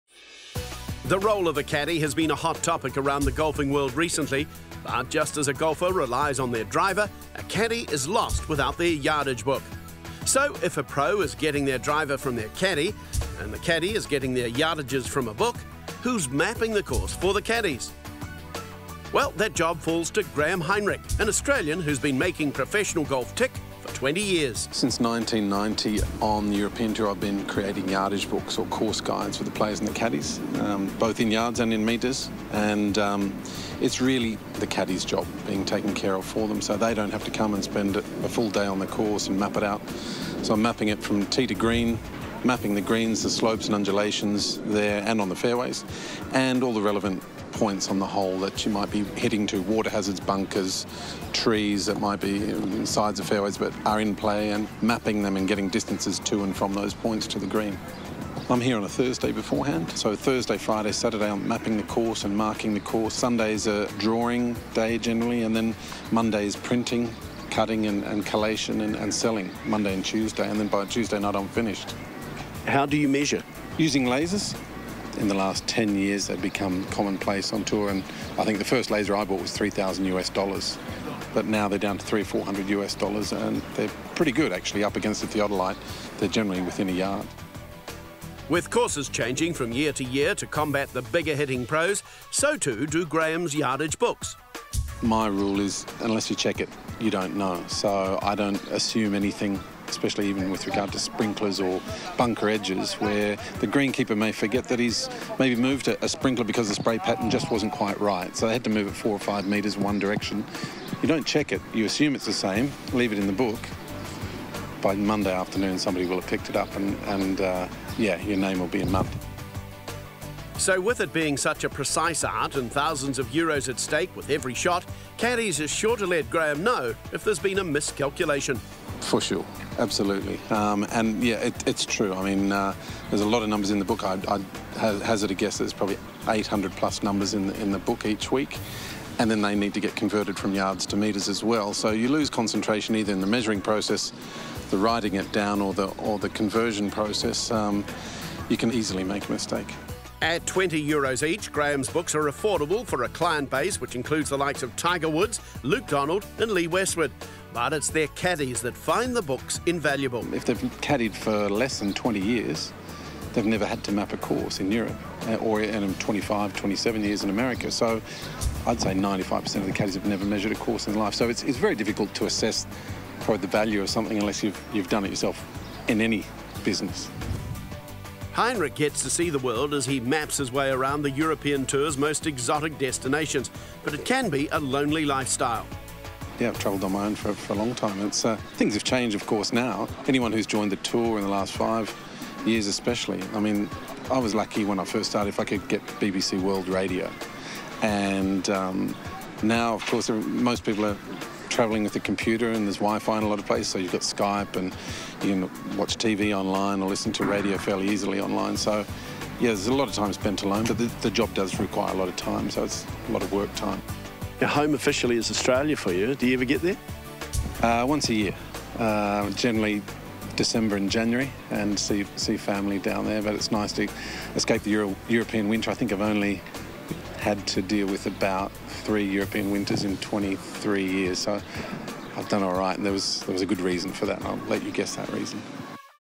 Graeme Heinrich :: European Tour Course Guides
With clients such as Tiger Woods, Lee Westwood and Luke Donald, designing course guides for the European Tour is a pressure job. Graeme Heinrich has been mapping the world's greatest golf courses for 20 years. - and it's a dream job for any golfing fanatic.